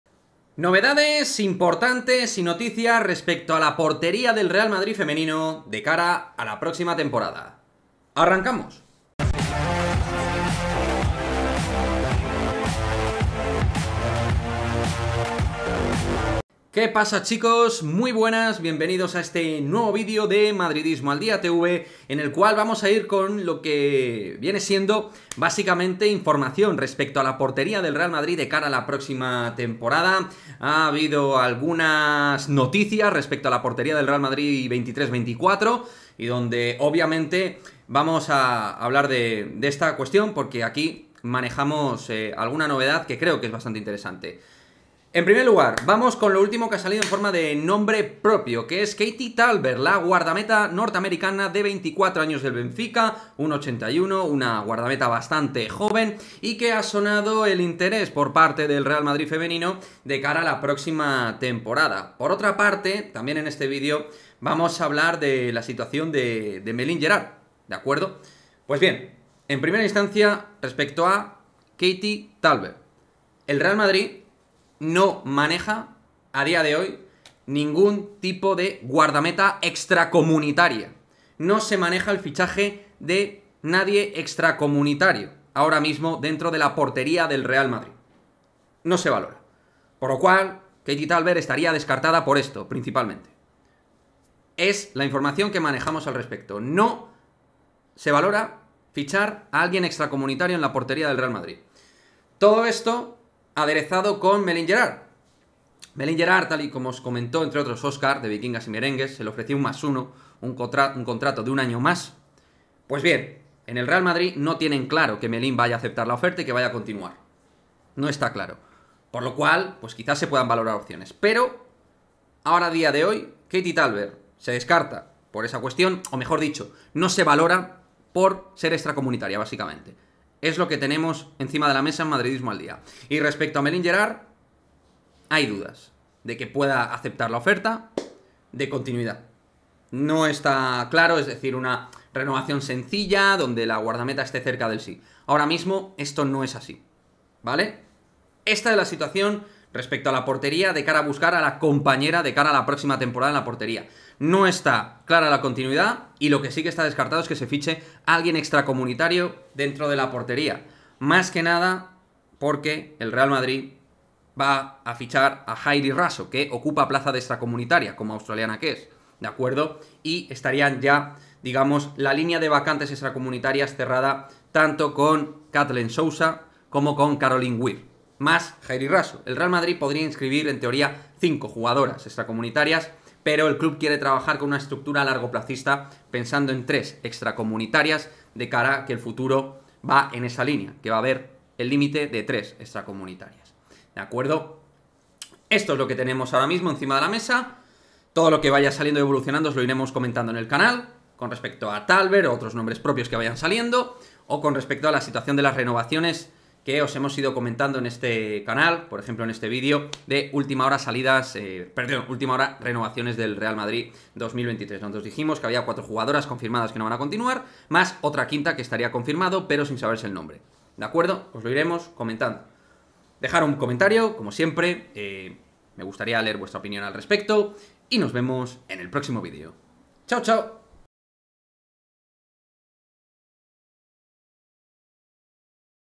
👩🟣 ÚLTIMA HORA de KATIE TALBERT | REAL MADRID 2023 ✅✍
Vamos con la última hora de Katie Albert, jugadora que ha sonado en las últimas horas para el Real Madrid.

El Real Madrid Femenino tiene pendiente distintas cuestiones y una de ellas puede tener que ver con la portería.

¿Katie Talbert y el Real Madrid es una opción?. Lo comentamos.